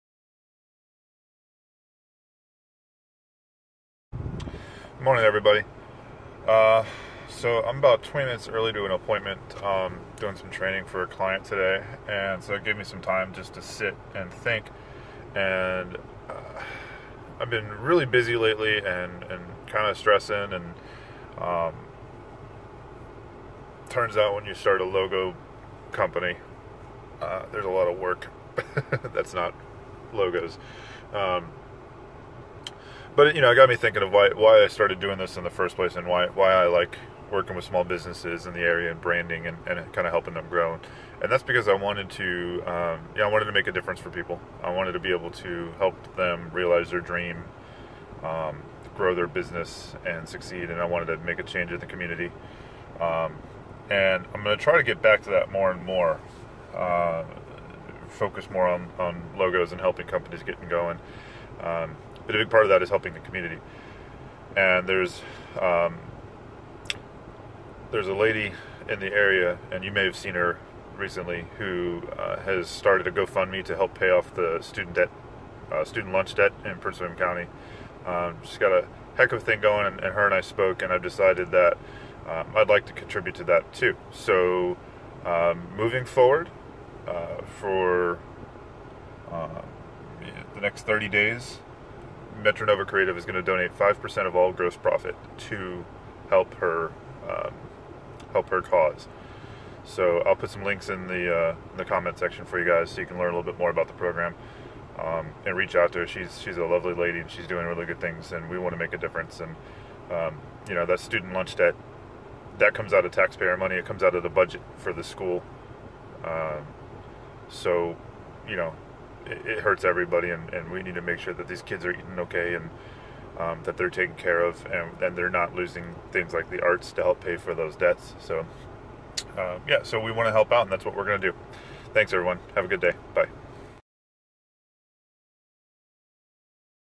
Helping our Community
Metro Nova Creative will donate 5% of our gross profit until July 12, 2017 to help pay off student lunch debt for Prince William County.